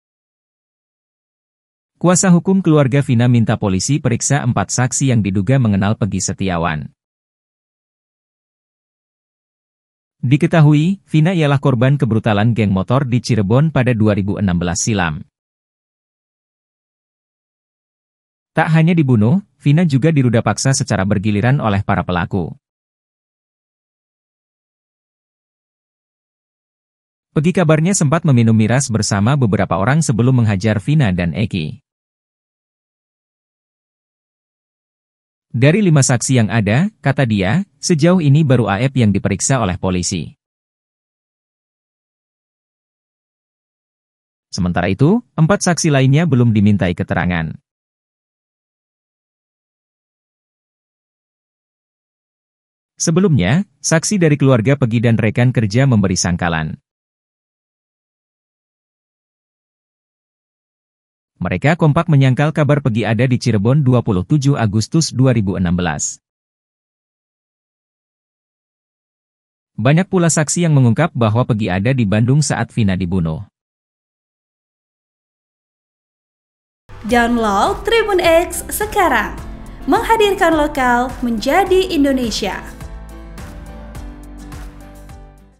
Kuasa Hukum Vina Minta Polisi Panggil 4 Teman Pegi, Sebut Sempat Minum Miras Sebelum Habisi Vina
TRIBUN-VIDEO.COM - Kuasa hukum keluarga Vina Cirebon meminta polisi memeriksa empat saksi yang diduga kenal dengan Pegi Setiawan alias Perong.

Vina merupakan korban kebrutalan anggota geng motor di Cirebon pada 2016. Bukan cuma dibunuh, Vina juga diperkosa bergiliran.

Selain Vina, Eki yang merupakan kekasihnya juga meninggal dalam peristiwa nahas itu.

Dea Saskia, satu di antara kuasa hukum keluarga Vina, mengatakan, Pegi diketahui sempat minum-minuman keras bersama sejumlah orang sebelum peristiwa pengeroyokan terhadap Vina dan Rizky alias Eky.

"Pegi minum-minuman ciu di warung Bu Nining. Mereka minum bersama saksi bernama Nurdatul Kahfi, Pramudia Wibawa Jati, Teguh Wijaya, Ahmad Saepudin atau Aep, dan Okta Rangga," ujar Dea di Mapolda Jabar, Bandung, Rabu (5/6/2024).

Dari lima saksi yang ada, kata dia, sejauh ini baru Aep yang diperiksa oleh polisi. Sedangkan empat saksi lainnya belum dimintai keterangan.

"Jadi, untuk menunjukkan Pegi pelaku atau bukan, ya saksi ini harus dipanggil, diperiksa. Kemungkinan mereka juga tahu Pegi ini pelaku atau bukan," katanya.

"Kalau didukung sama keterangan saksi lainnya yang minum bersama saat sebelum kejadian, kemungkinan lebih akurat lagi. Mereka ini minum bareng sama Pegi, tapi tidak turut terlibat sebagai pelaku. Mereka tidak melakukan pengeroyokan," ucap Dea.

Sebelumnya, sejumlah saksi dari keluarga Pegi dan rekan kerjanya sesama kuli bangunan, menyangkal bahwa Pegi berada di Cirebon pada 27 Agustus 2016 atau saat kejadian Vina dan Eky dikeroyok sejumlah orang.

Pegi ditangkap delapan tahun setelah peristiwa terjadi.

Dia diamankan di Kopo, Bandung, Selasa (21/5/2024).

Pegi disebut sebagai satu di antara tiga buron kasus Vina Cirebon. Bahkan saat menetapkan sebagai tersangka, polisi menyebut Pegi sebagai otak kejadian.

Namun, banyak saksi mengungkap, Pegi di Bandung saat Vina dan Eky dianiaya hingga meninggal dunia.

Artikel ini telah tayang di TribunJabar.id dengan judul Keluarga Vina Cirebon Minta Polisi Panggil 4 Teman Minum Pegi Sebelum Kejadian, Baru Aep Diperiksa,
Penulis: Nazmi Abdurrahman | Editor: Giri

Editor Video: Yonatan Krisna Halman Tri Santosa